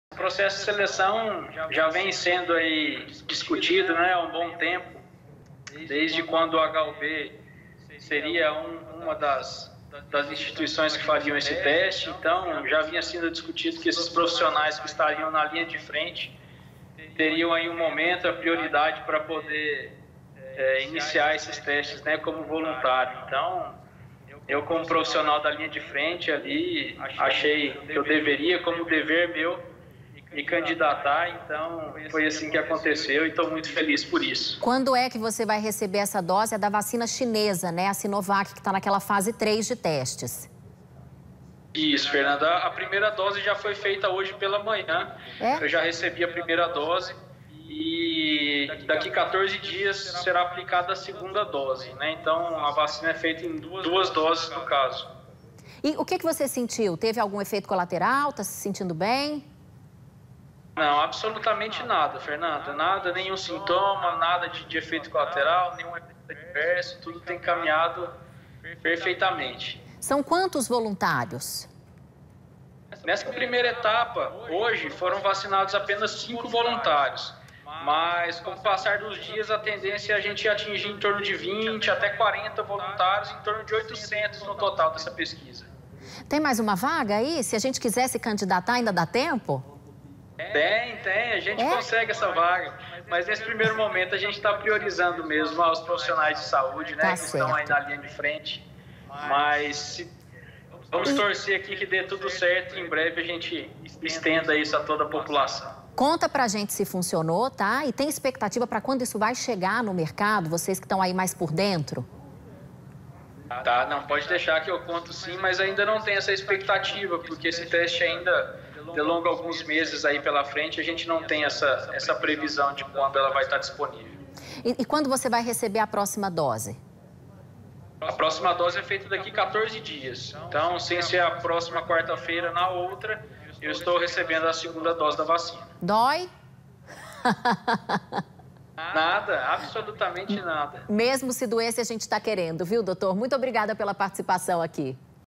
VACINA COVID -19: MÉDICO GOIANO VAI SER VACINADO
Médico goiano vai ser voluntário na vacinação contra o covid 19. Falamos pela internet com o médico Gabriel Ravazzi.